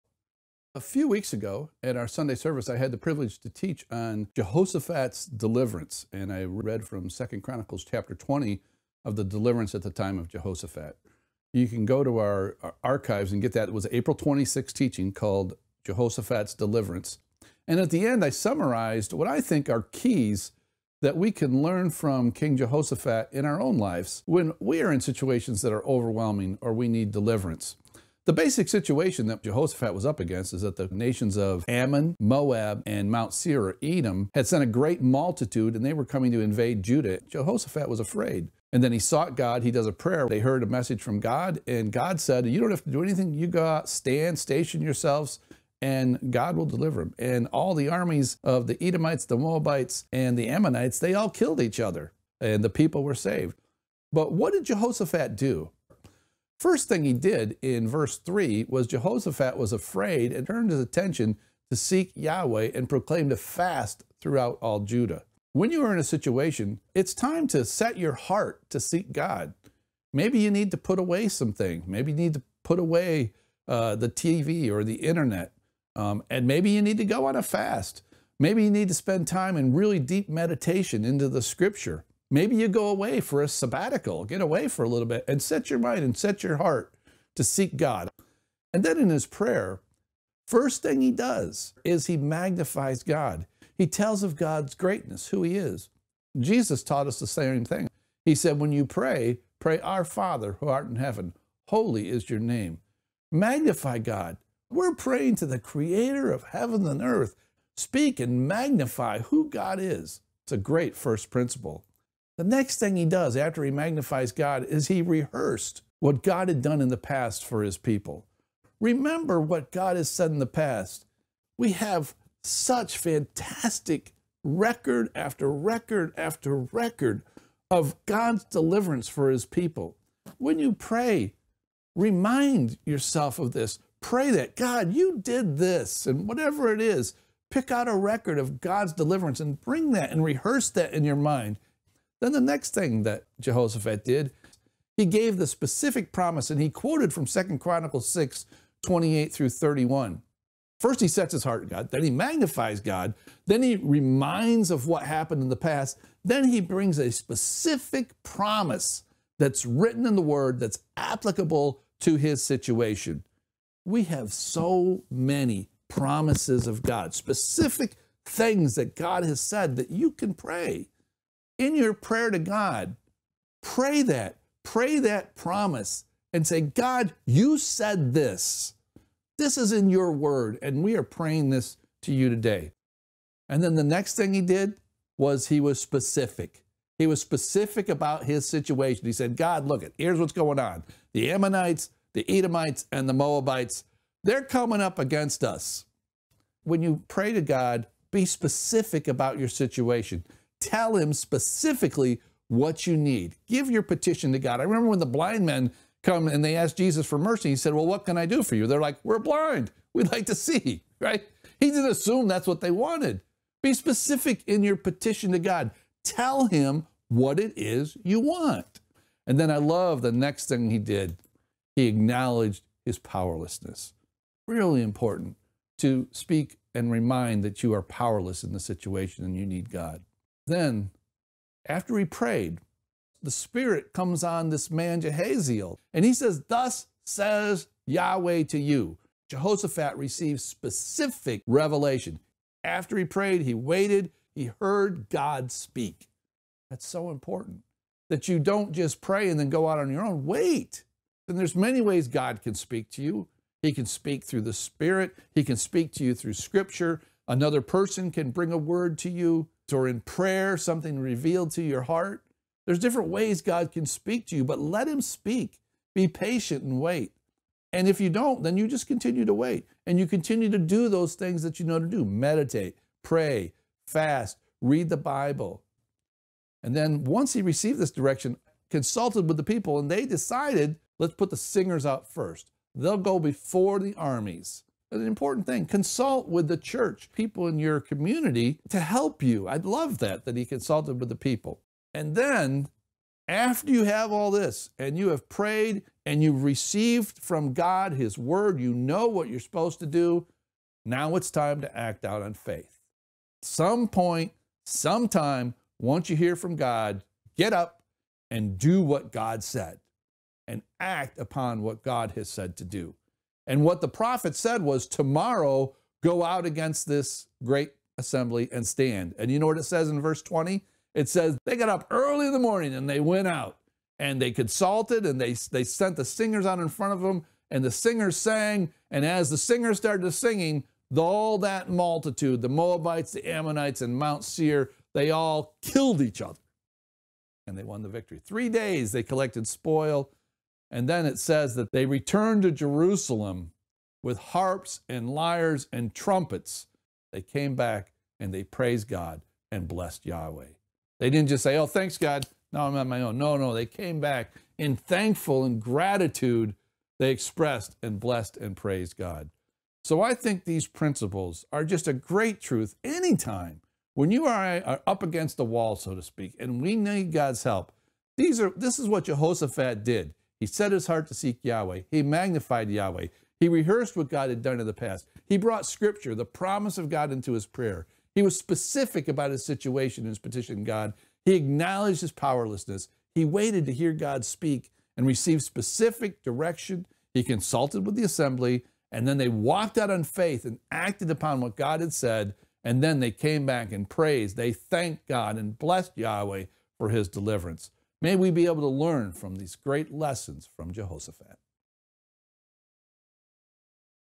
Lessons From Jehoshaphat
On April 26th the full Sunday Service Teaching was on “Jehoshaphat’s Deliverance.”  The teaching ended with a summary of the godly actions of the King.  Here again, John Cortright gives a short synopsis of these “Lessons from Jehoshaphat.”